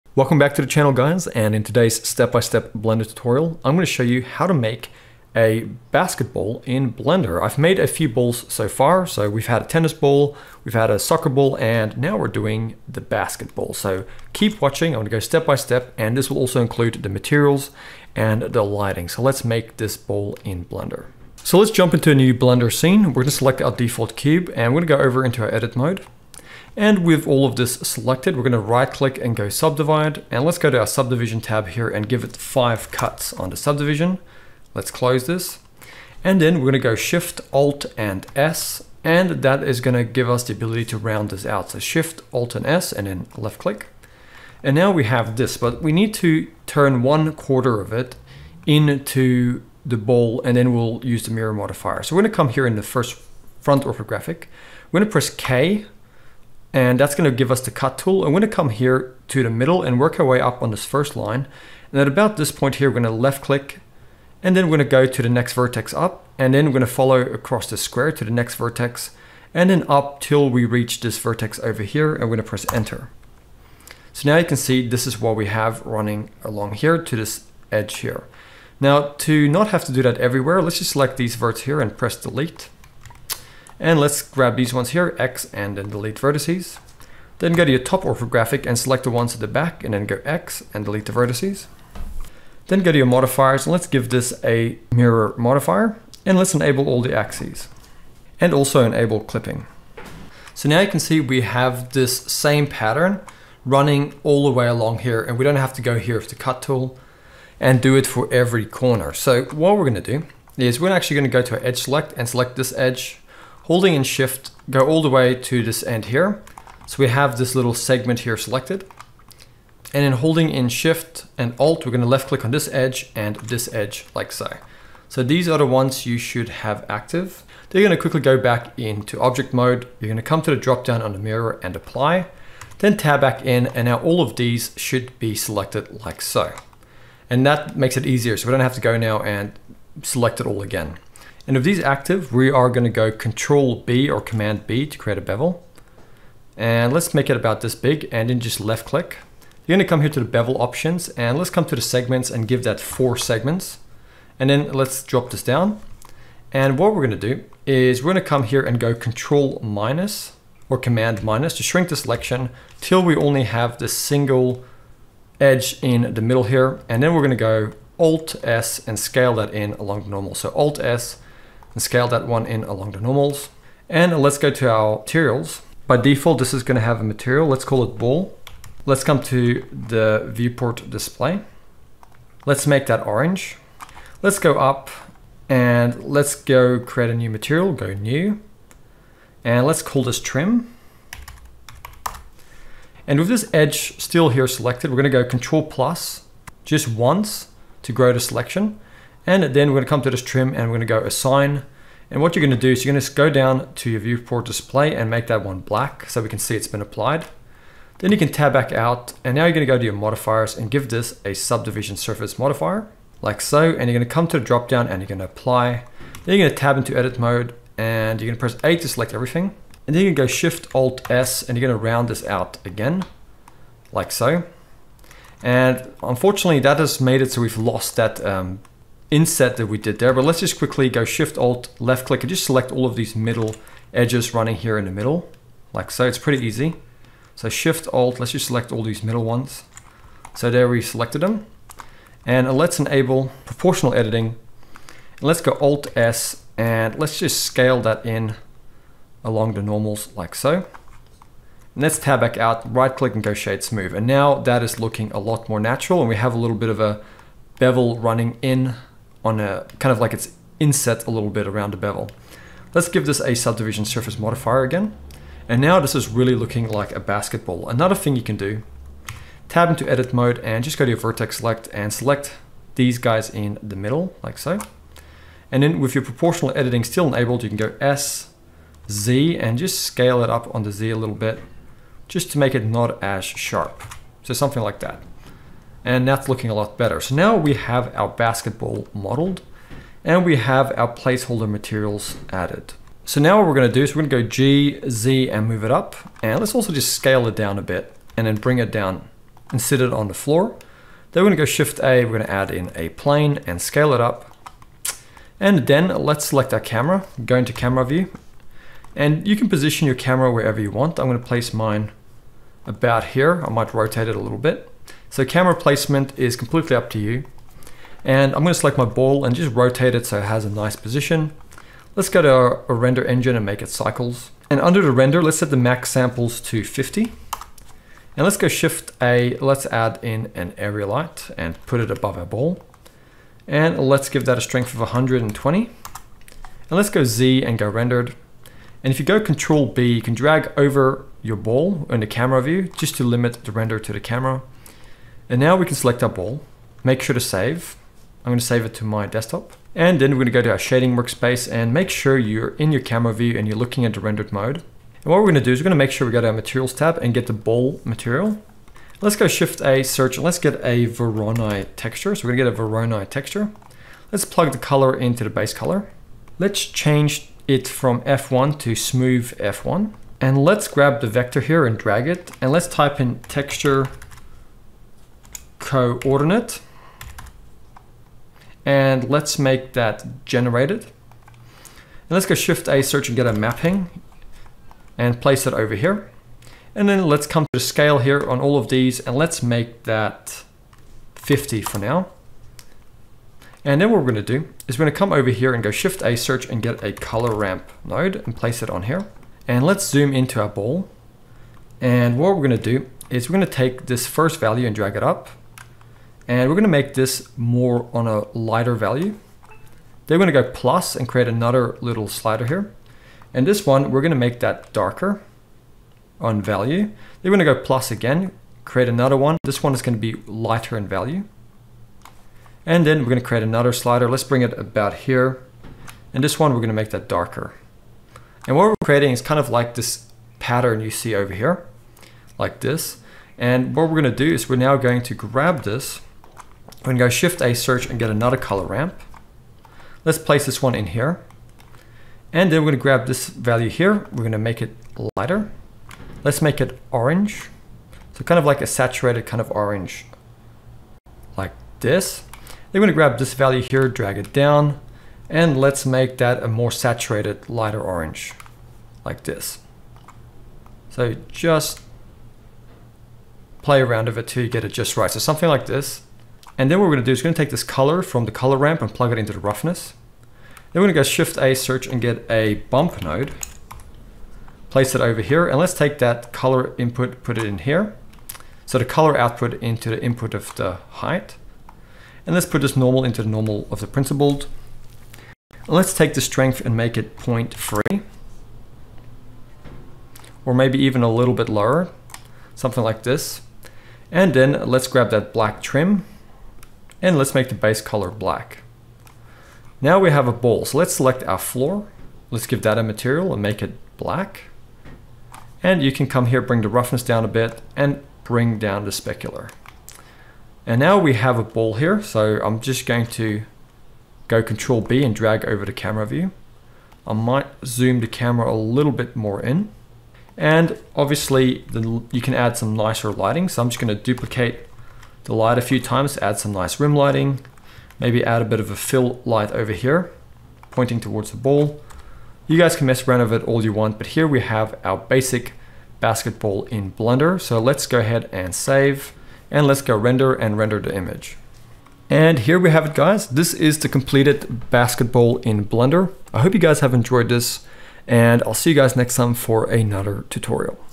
Blender Tutorial: Make A 3D Basketball | Beginners
Learn how to make a basketball in Blender.